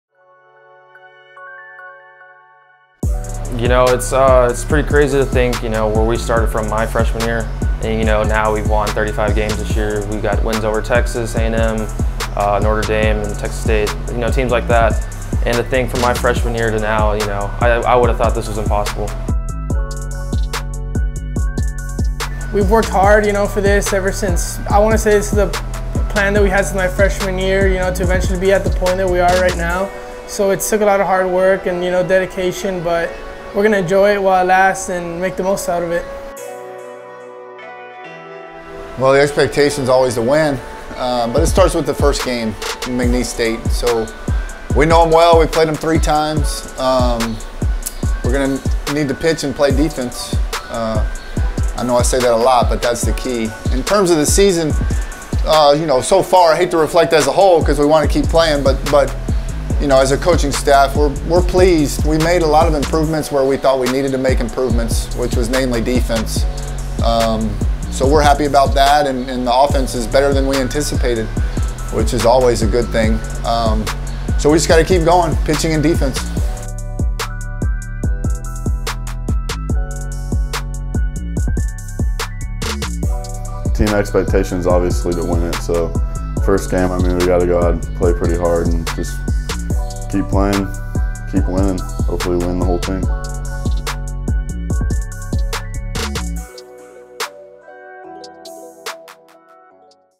UIW BASEBALL⚾️ 2019 Southland Conference Tournament
The University of the Incarnate Word baseball team earned the fourth seed for the Southland Conference Tournament, announced the league office Saturday. The Cardinals are slated to face fifth-seeded McNeese on Wednesday at 7 p.m. (CT) at Constellation Field in Sugar Land, Texas.

Every game of the 2019 Southland Conference Baseball Tournament will be available exclusively on ESPN+.

This is the first time the Cardinals have earned a spot in the postseason since joining Division I. UIW finished the regular season with a 35-20 overall mark and an 18-12 record in conference action.

UIW faced McNeese on March 22-24 in Lake Charles, Louisiana, with the Cardinals sweeping the three-game series. In the tournament this week, the winner of UIW's game four is scheduled to face the winner of No. 1 Sam Houston and No. 8 Texas A&M-Corpus Christi on Thursday at 7 p.m.